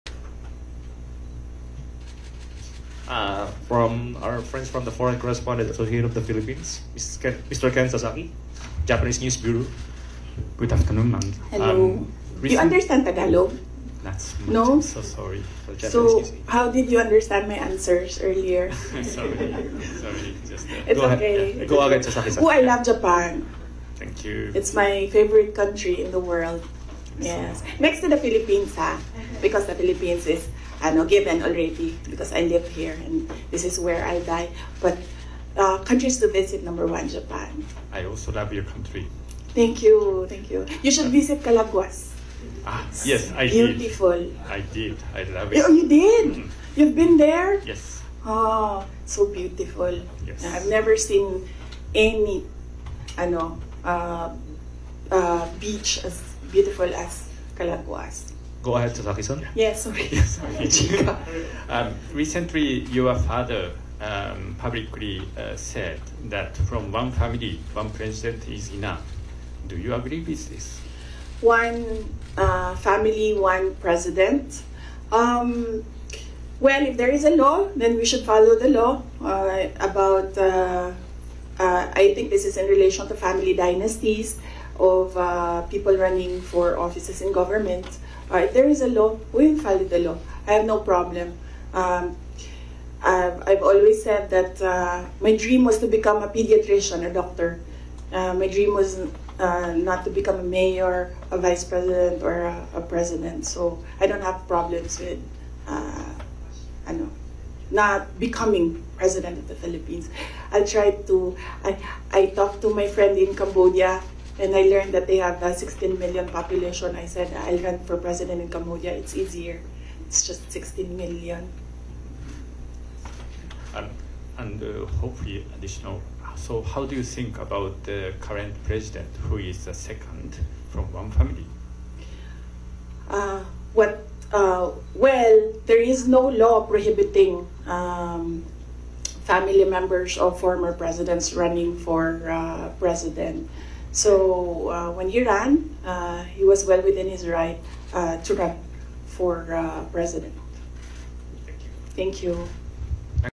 In light moment, Sara Duterte shares 'chika' with Japanese journalist at presscon | ABS-CBN News
In a light moment during her press conference on Wednesday (September 25), Vice President Sara Duterte talked travel destinations with a journalist from Japan, which she described as her favorite country next to the Philippines.

"Sorry, chika!" a laughing Duterte said, when the moderator finally prompted the journalist to ask his question after their chit-chat.